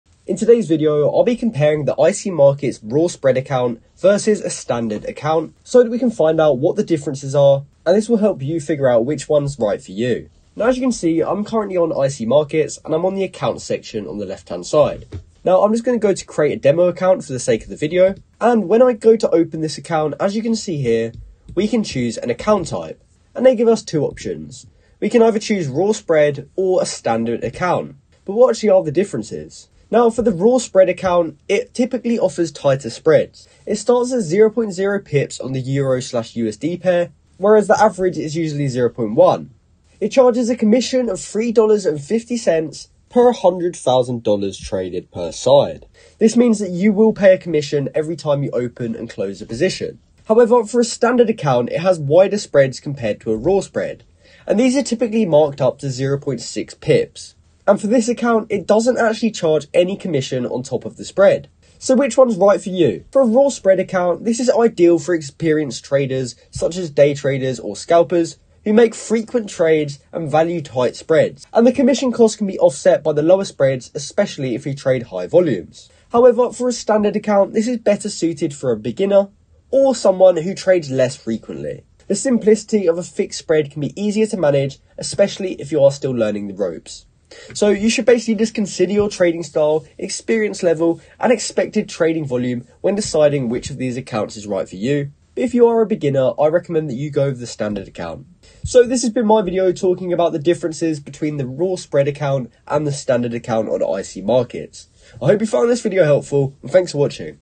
IC Markets Raw Spread vs Standard Account: Whats The Difference? (2024)
In today's video i will compare ic markets raw spread vs standard account so that you know the differences between them.